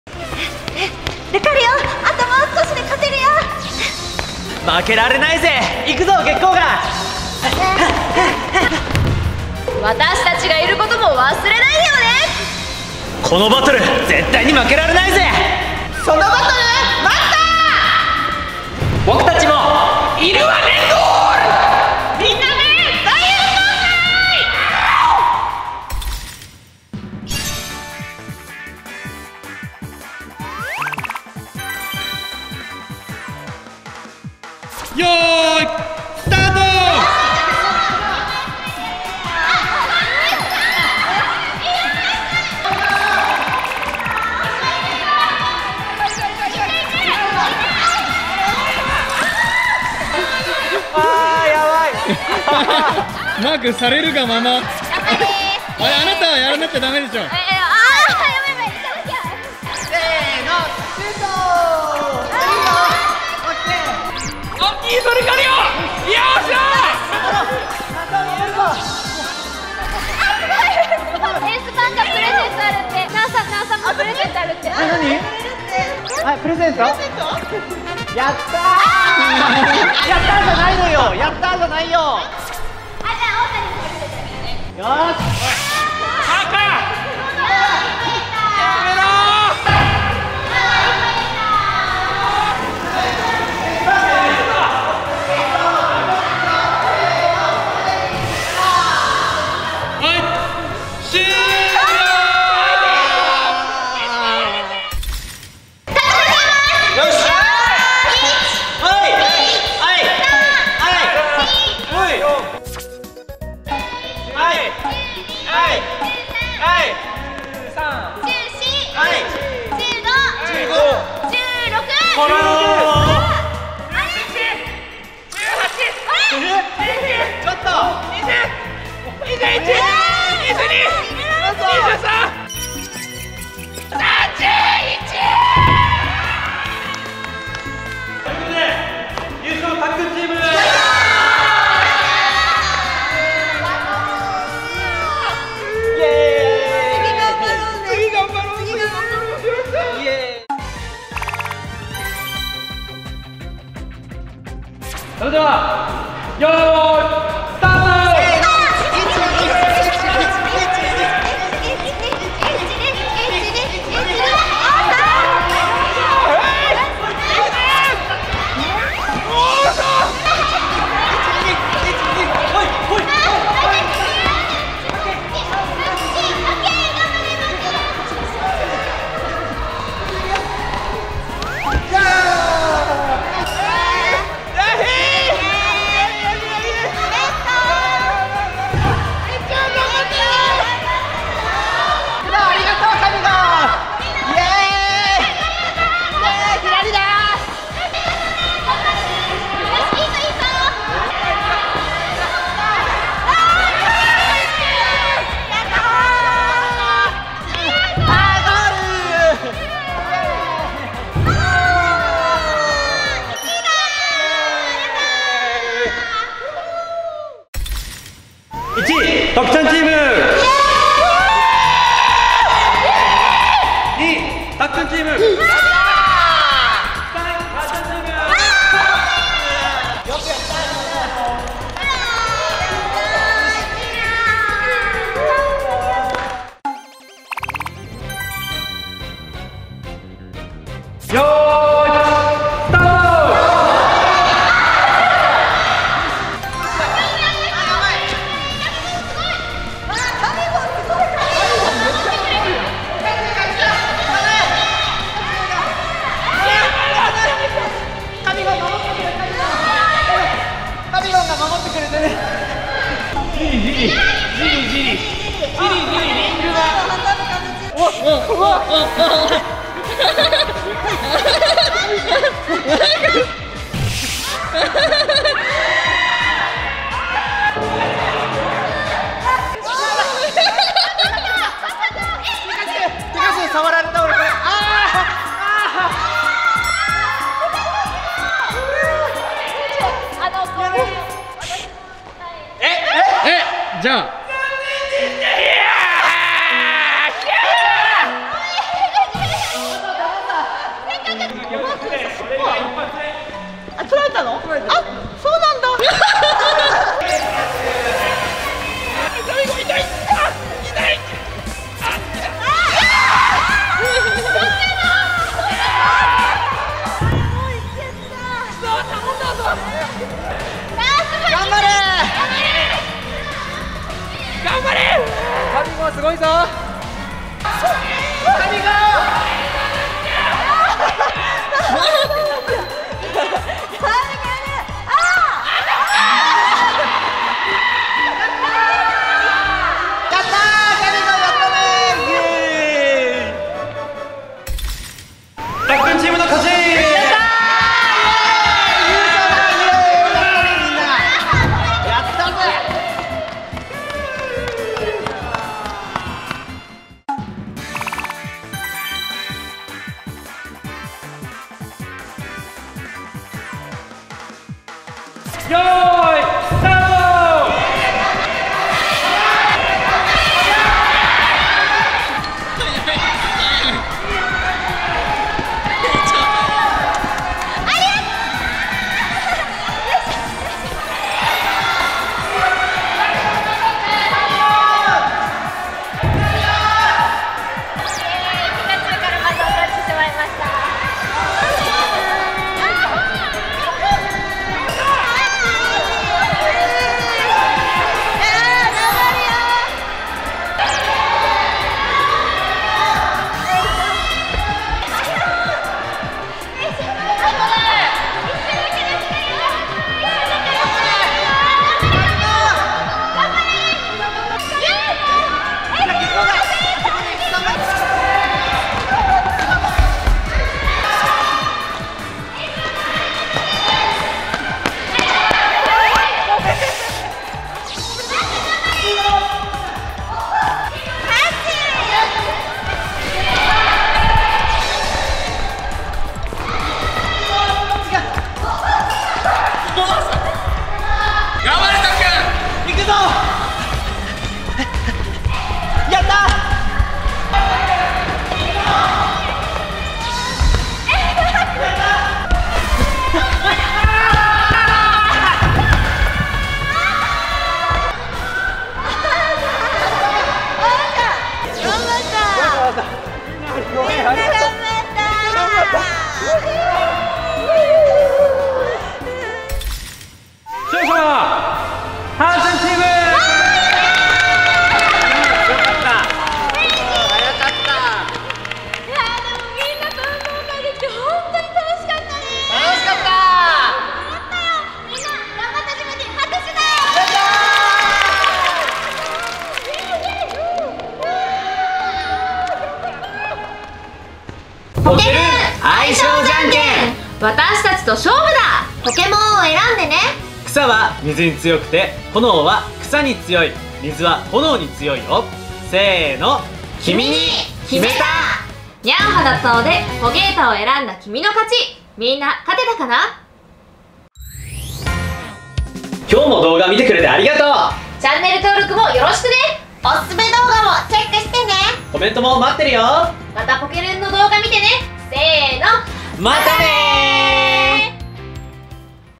体育館貸し切ってポケモンたちと大運動会！リレーや玉入れで大盛り上がり！ピカチュウ vs カビゴン全力リレー対決【学校】
【公式】『ポケットモンスター スカーレット・バイオレット』HP